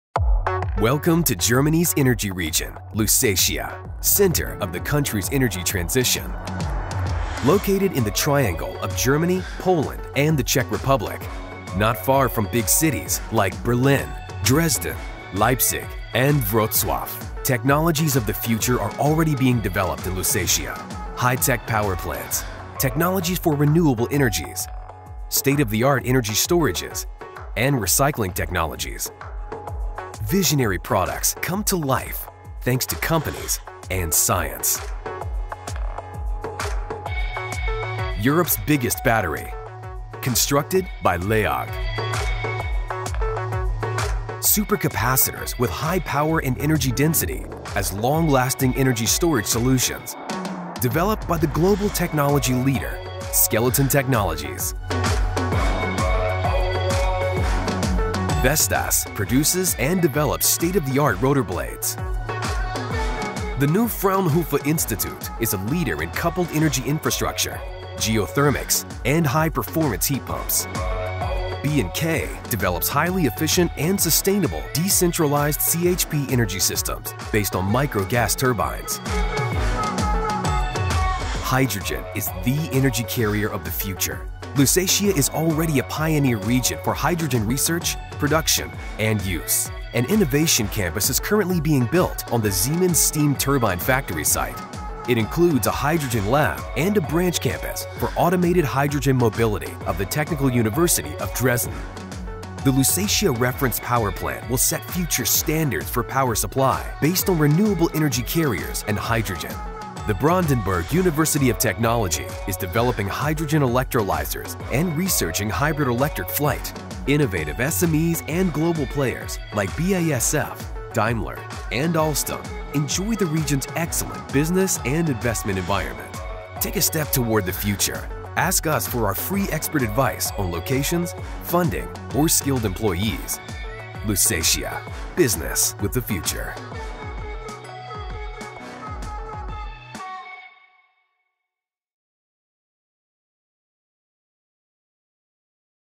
Lusatia – Germany´s pioneering energy region
Lusatia is THE center of Germany´s energy transition as technologies of the future are already being developed here today:
- High tech power plants
- Recycling technologies
- State-of-the-art energy storages
- Technologies for renewable energies.
Visionary products are brought to life thanks to companies and science.

„Die Lausitz – Deutschlands zukunftsweisende Energieregion“
Die Lausitz ist DAS Zentrum der Energiewende in Deutschland, denn hier werden schon heute die Technologien der Zukunft entwickelt:
- Hightech-Kraftwerke
- Recycling-Technologien
- Modernste Energiespeicher
- Technologien für erneuerbare Energien
Visionäre Produkte werden dank Unternehmen und Wissenschaft zum Leben erweckt.